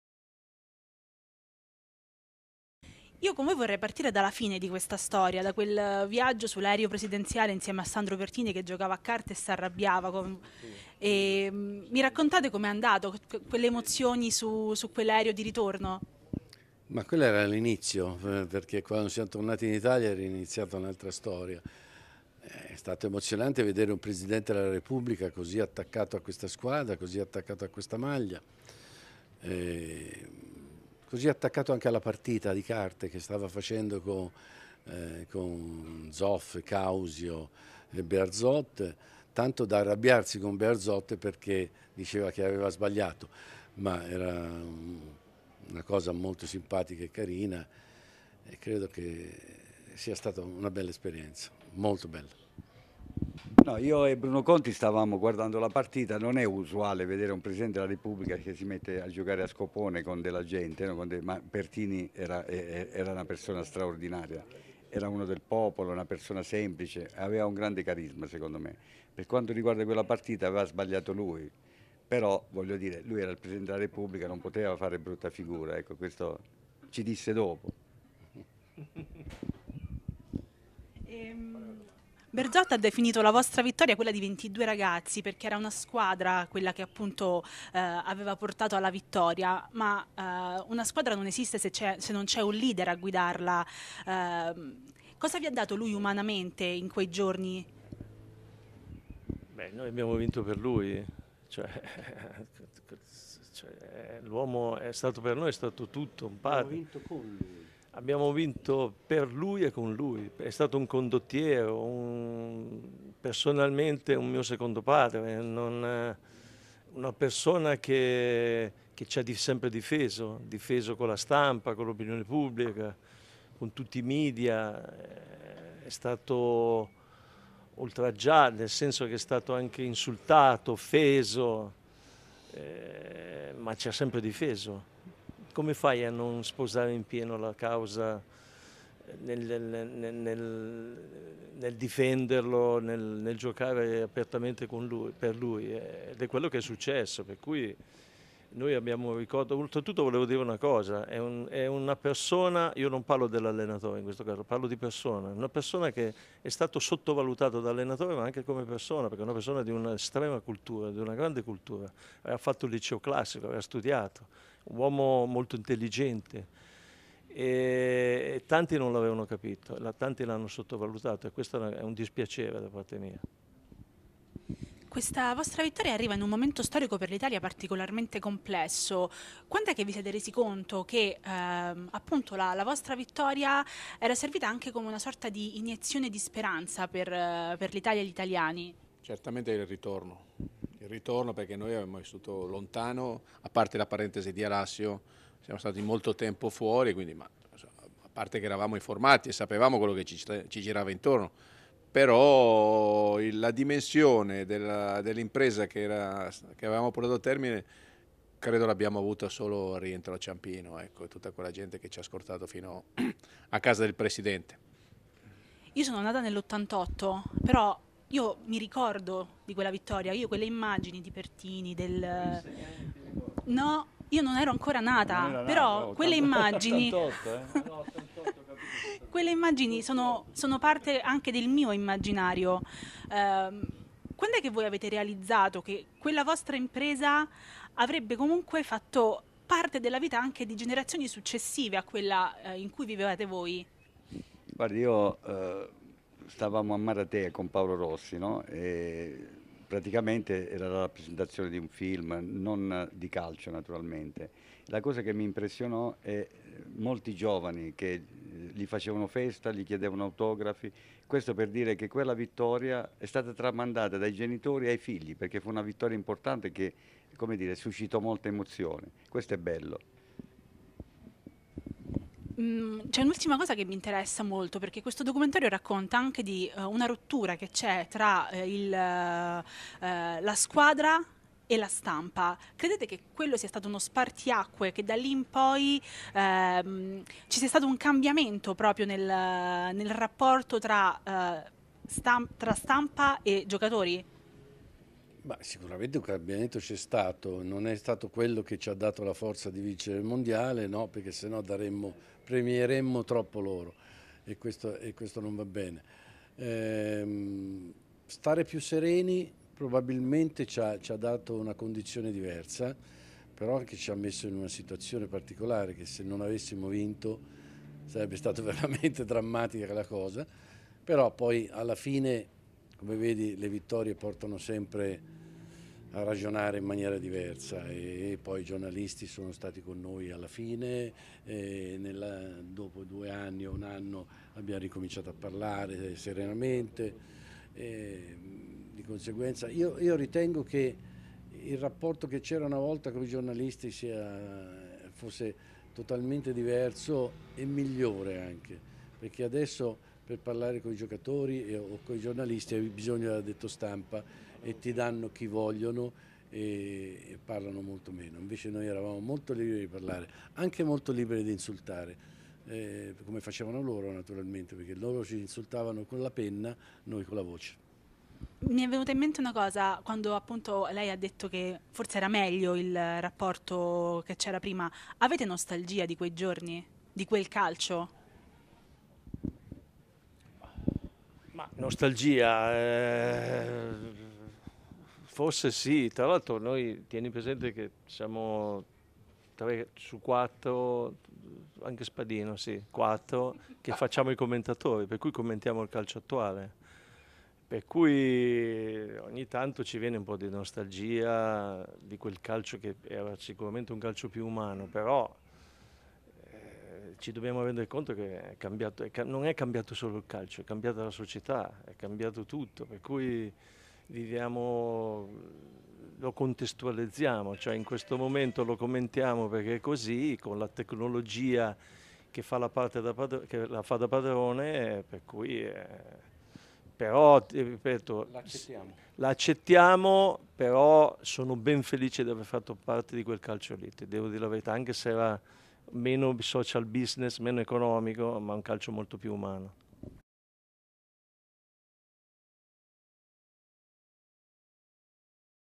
ITALIA 1982 - UNA STORIA AZZURRA | Intervista a Tardelli, Collovati, Dossena, Selvaggi | HOT CORN
Fulvio Collovati, Beppe Dossena, Franco Selvaggi e Marco Tardelli raccontano il documentario Italia 1982 - Una Storia Azzurra di Coralla Ciccolini. Al cinema l'11-12-13 luglio.
Intervista a cura di Manuela Santacatterina